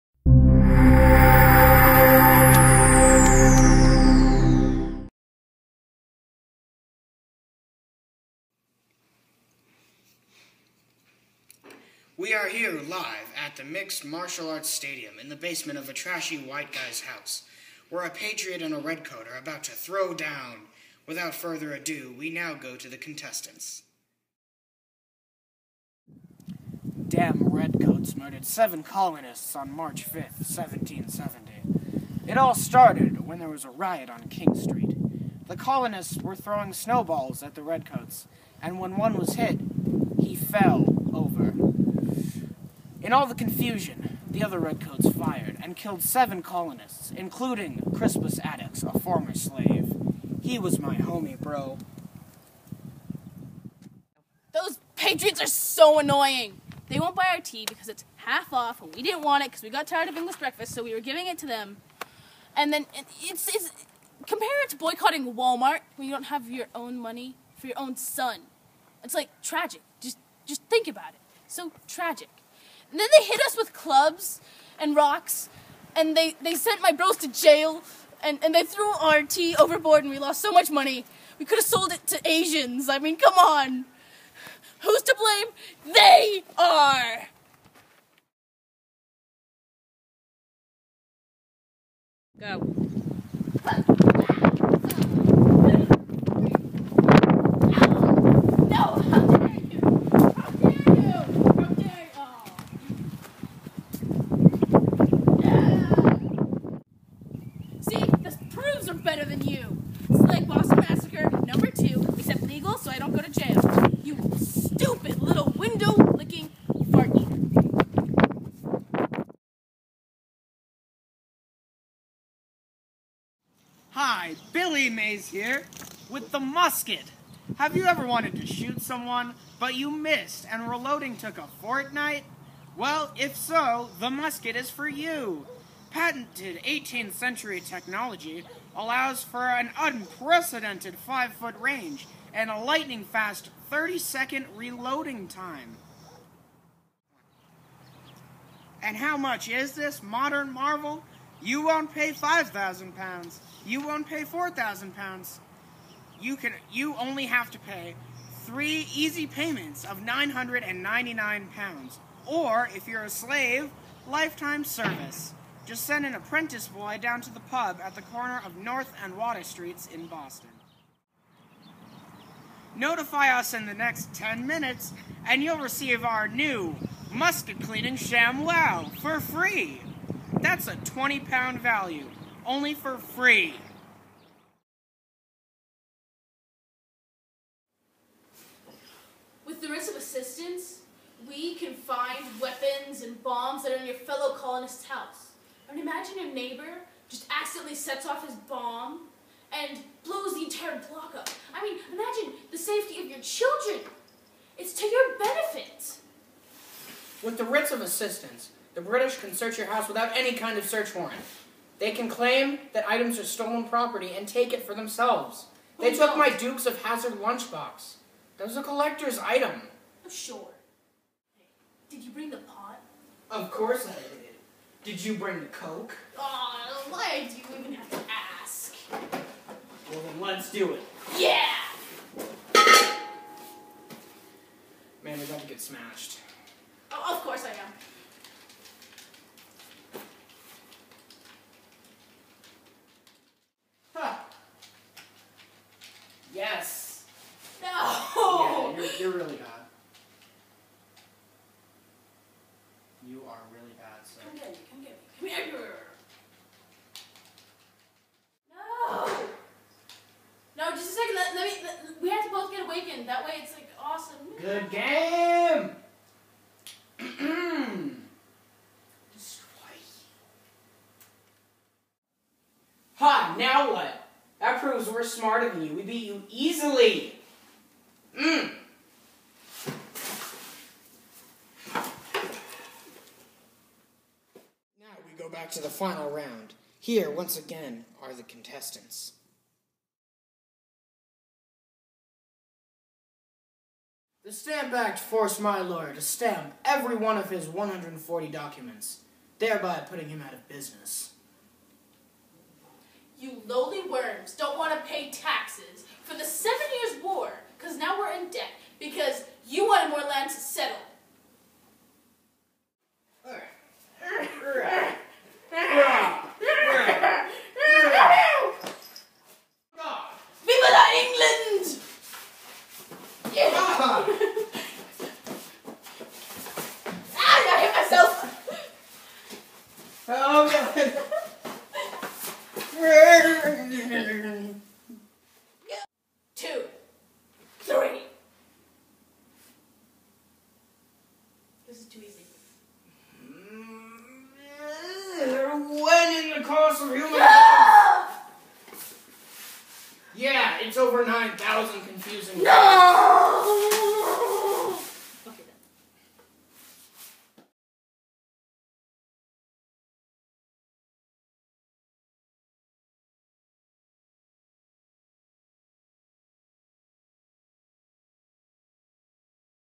MMA Fight: Boston, 1775
Lucari and Evan wrote the script for, and performed, this short film for Mr. Narvaez's 8th grade Social Studies class at Lynn Middle School.